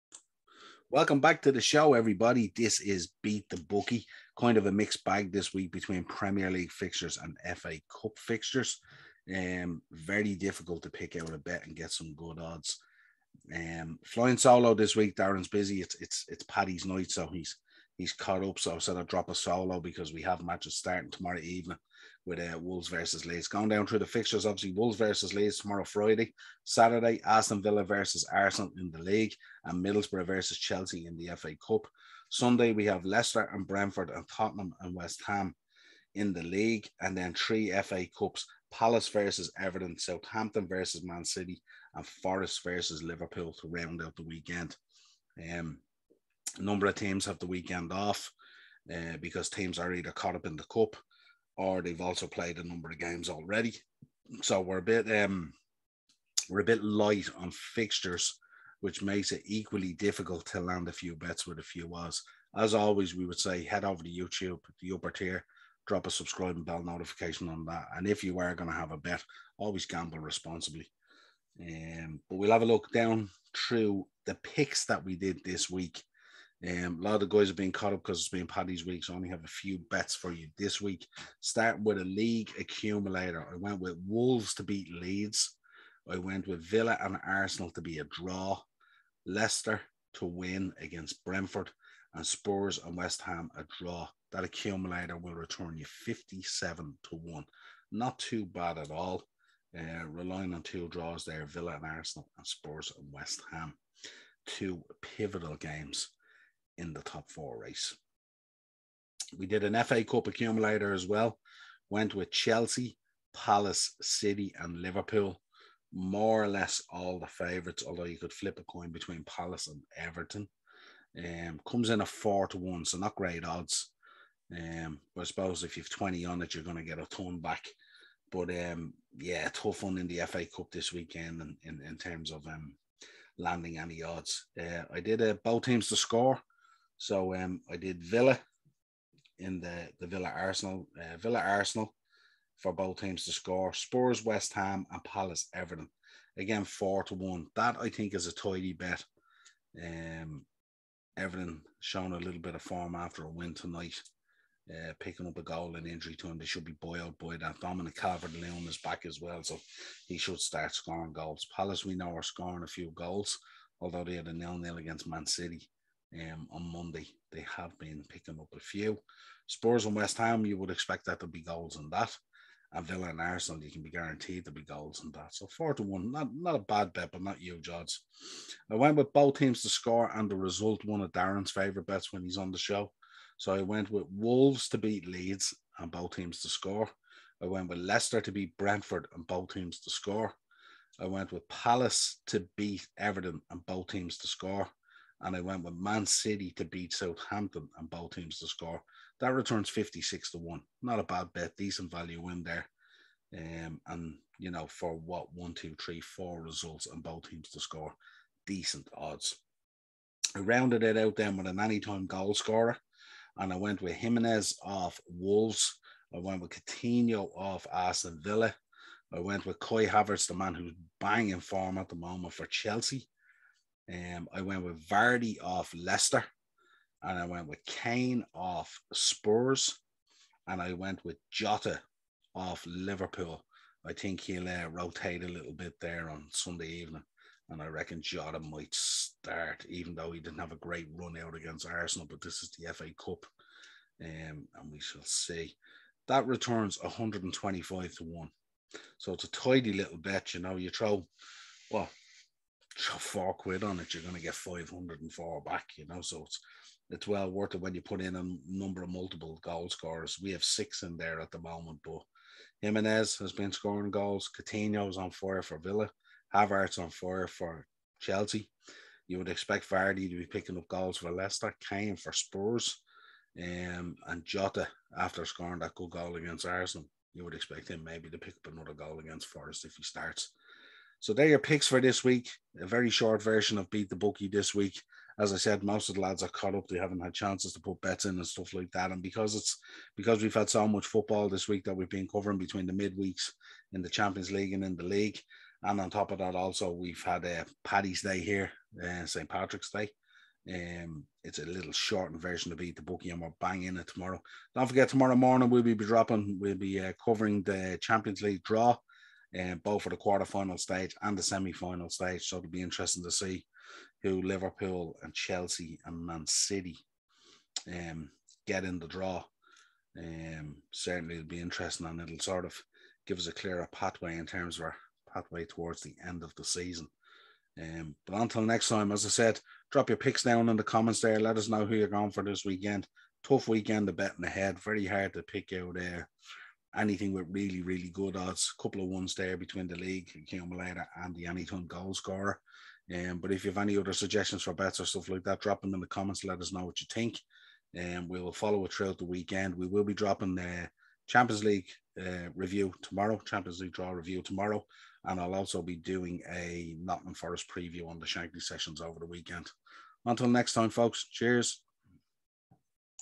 Beat The Bookie Premier League/ FA Cup
Niall drops his bets and tips for this weekend's round of fixtures in the PremierLeague &